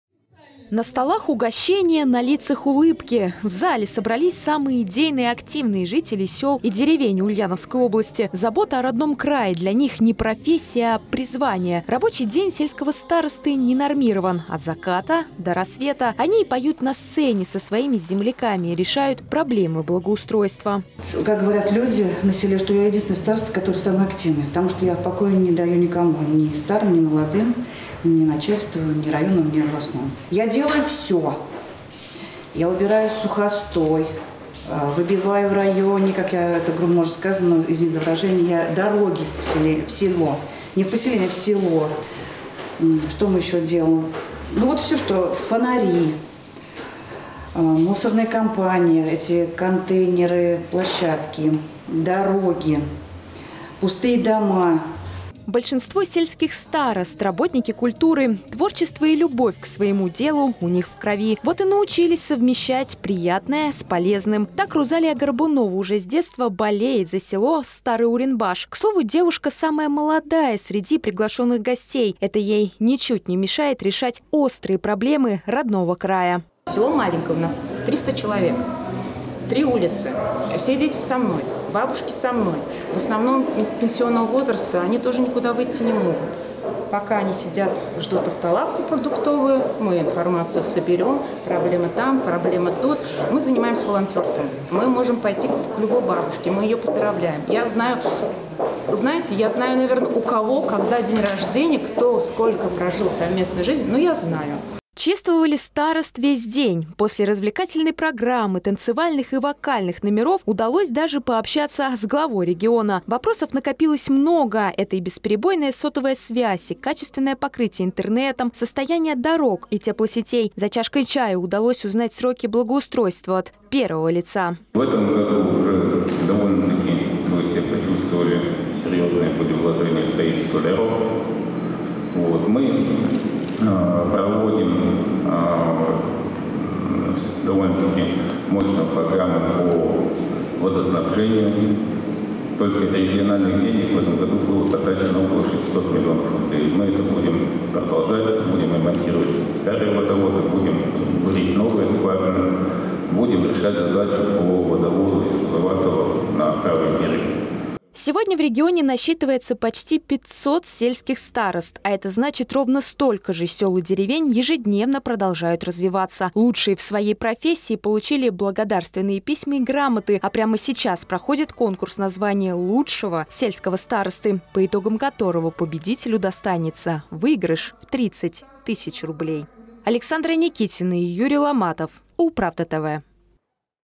От заката до рассвета в деле. Зачем на селе нужны старосты
В Ульяновской области отметили день сельских старост. Их полномочия и статус отражены в одноименном региональном законе. Он принят в 2011 году и помог области одной из первых создать Институт сельских старост. С этого момента и пошло чествование активистов. На празднике, помимо первых лиц региона, побывала и наша съемочная группа.